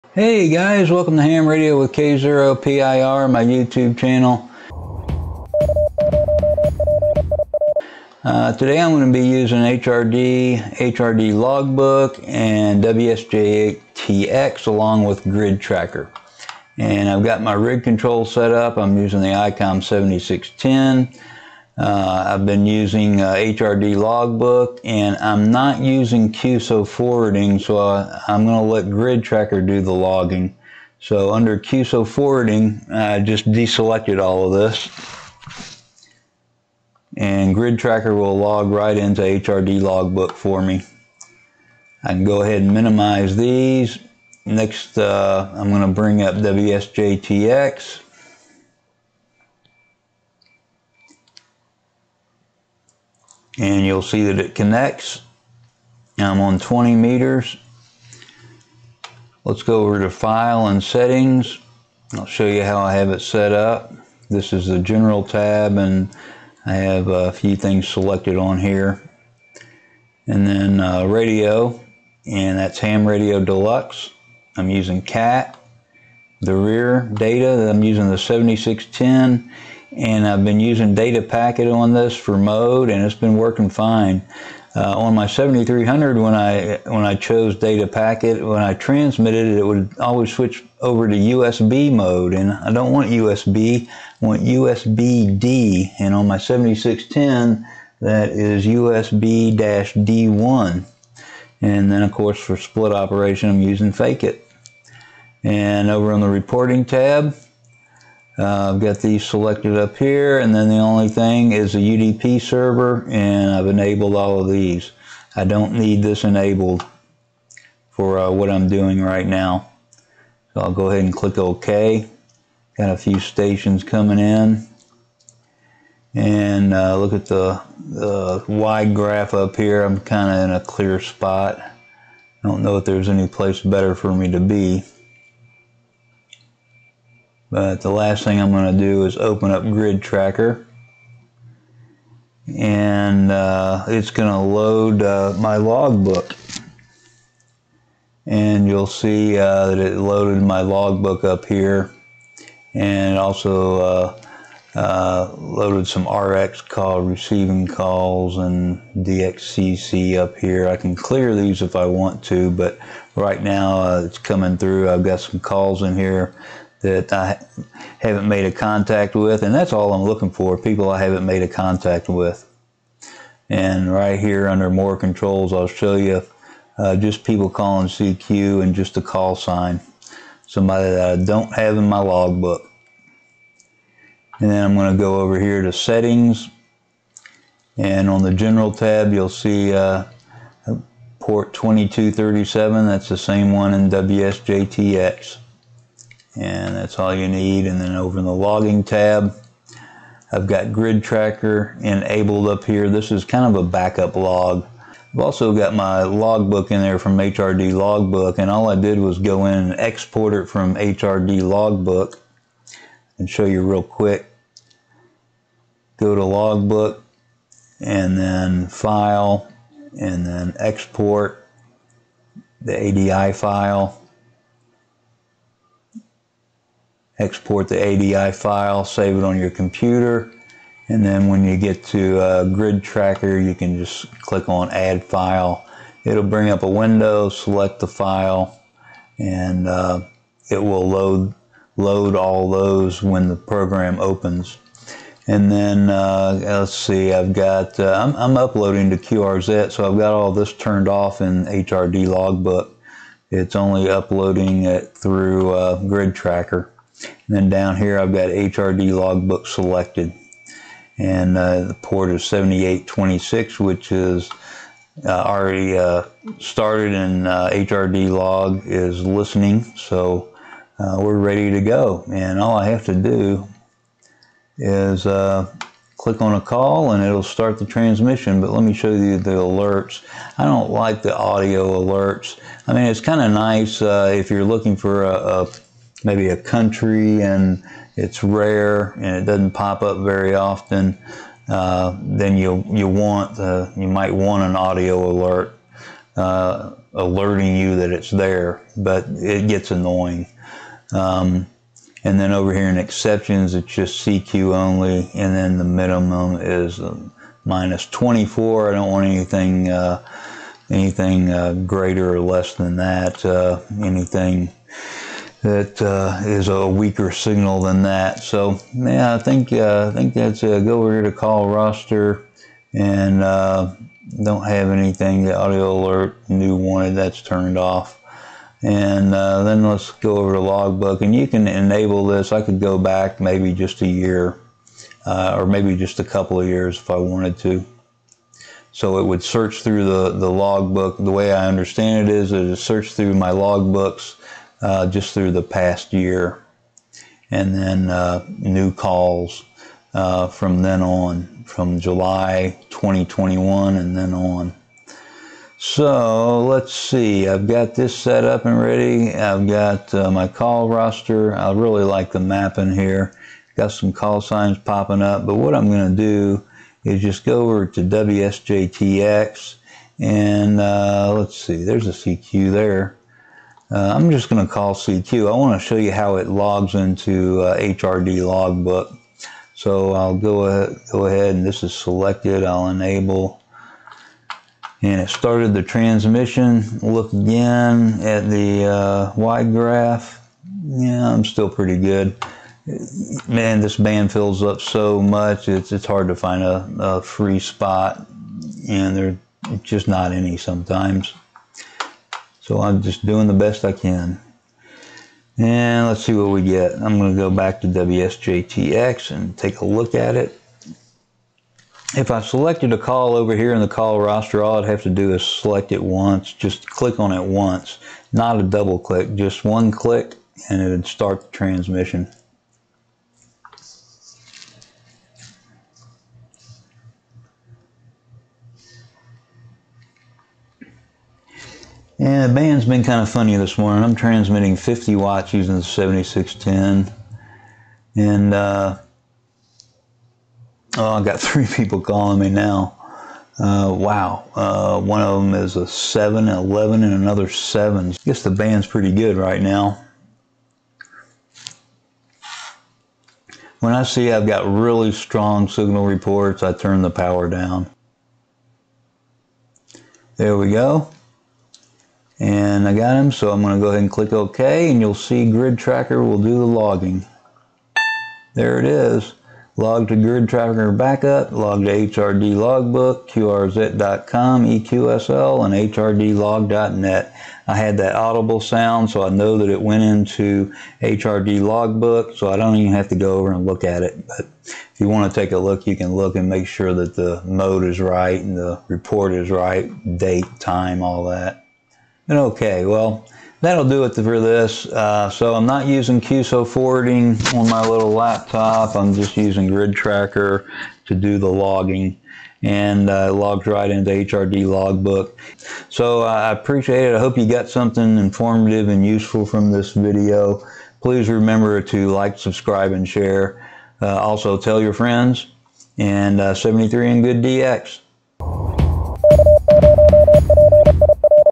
HRD QSO Forwarding Alternative Using GridTracker
Here’s a slick way to bypass QSO forwarding and let GridTracker do the work. Also, GridTracker can do the heavy lifting when it comes to uploading to online logs like eQSL, QRZ.com and HRD.net. Here’s how…

○○○ LINKS ○○○